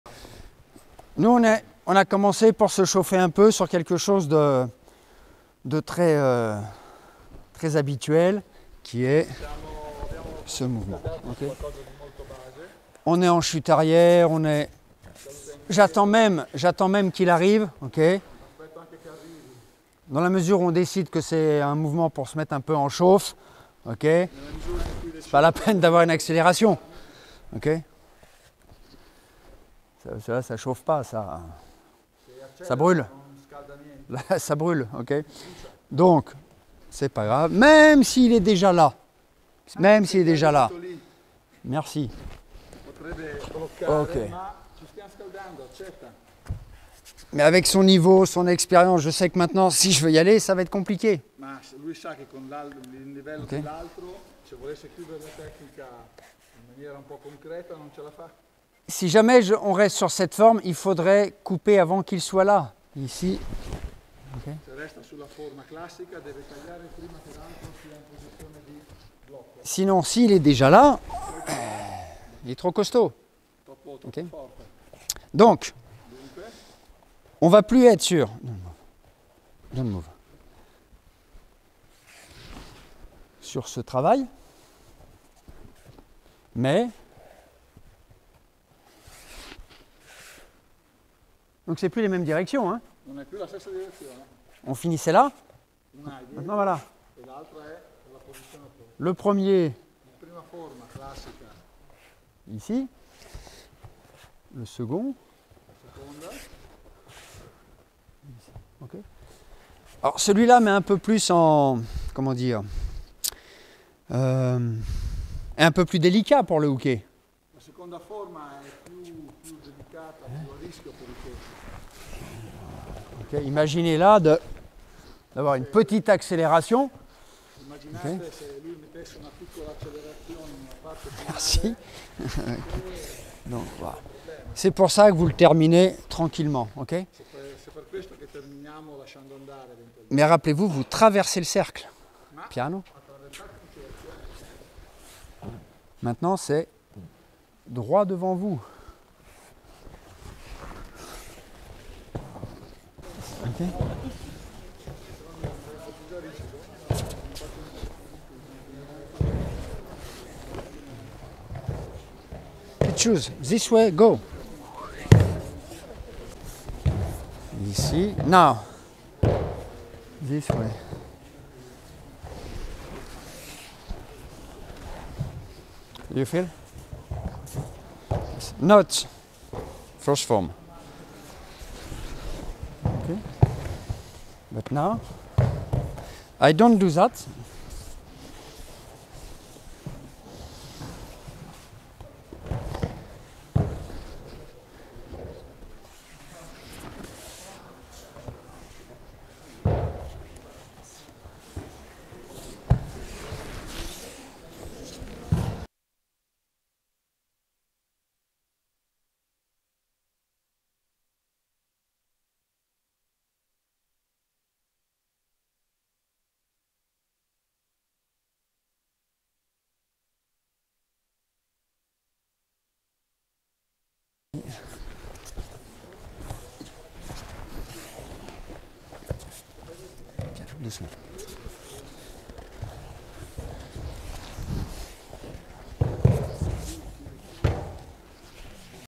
Pascal Guillemin: seminar in Brescia '23 - Shihonage
In questo video, sensei Pascal Guillemin ci offre una versione della tecnica di shihonage che ci permette di evitare che uke si posizioni in modo tale da annullare la tecnica.